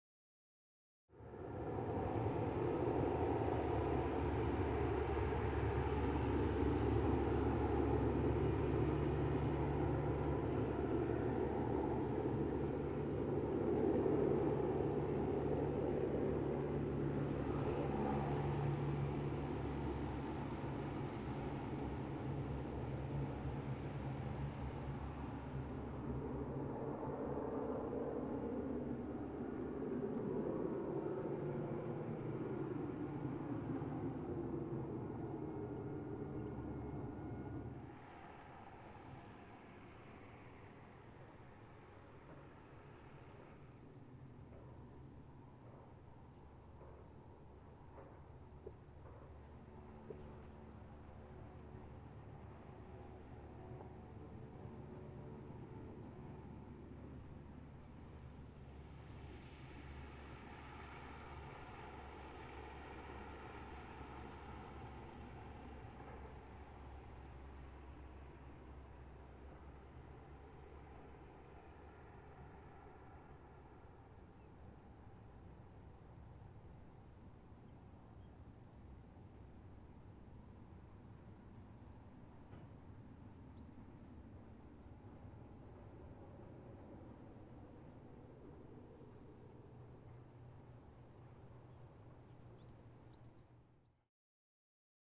航空自衛隊 E-2C(54-3458) 那覇基地 2015年12月14日
2015年12月14日に瀬長島でHC-X1000を使い撮影した航空自衛隊 E-2C(54-3458)の動画です。